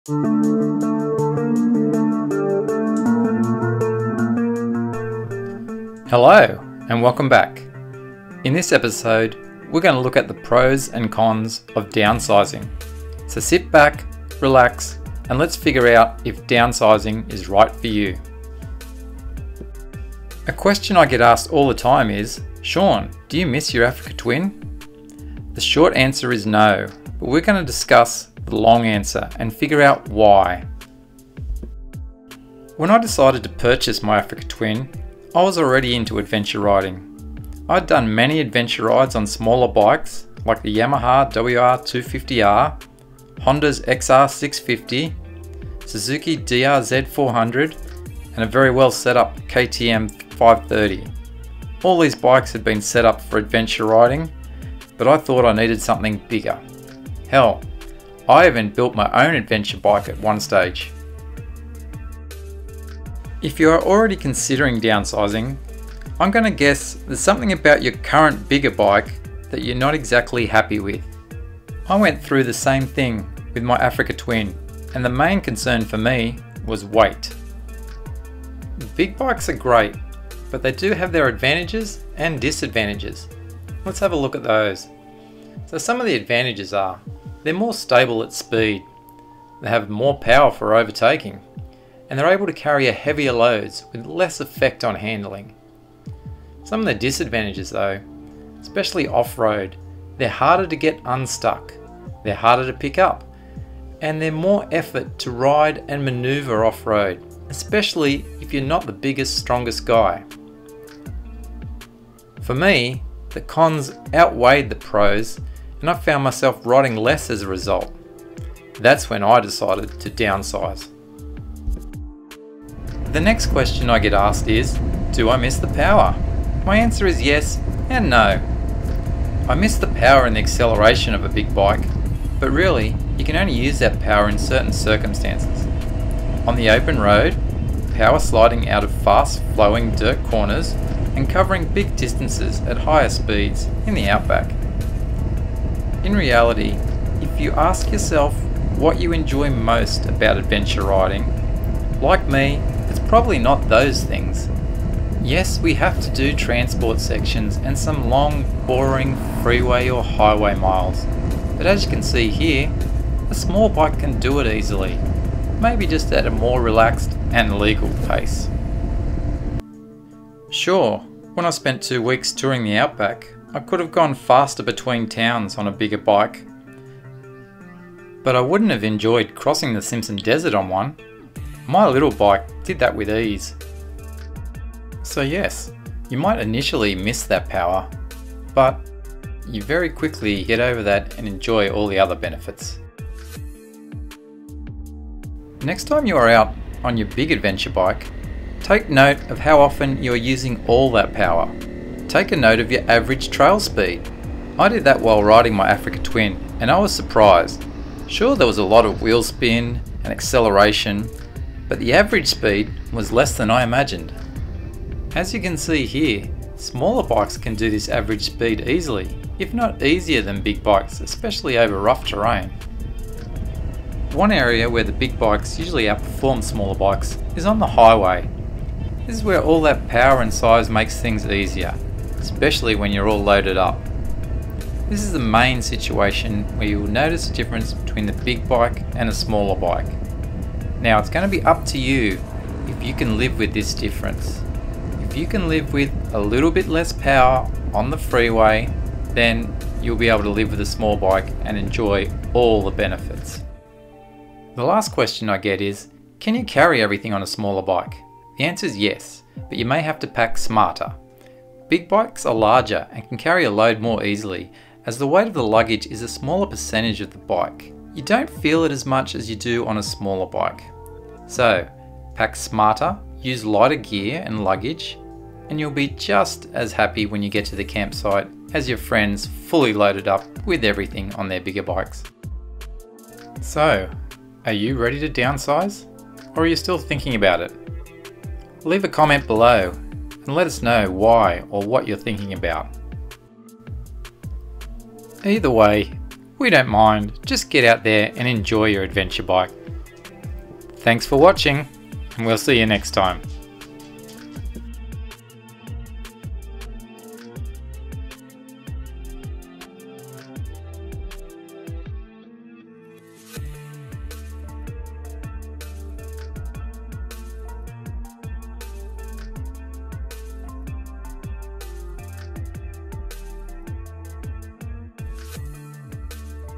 Thinking about downsizing? This may help.
I answer three simple questions about downsizing:

1 -  Do I miss my Africa Twin (or any large adventure bike)?

2 - Do I miss the power?

3 - Can you still carry all your camping gear on a smaller bike?

If you are thinking about downsizing your bike, maybe this video will help you with your decision...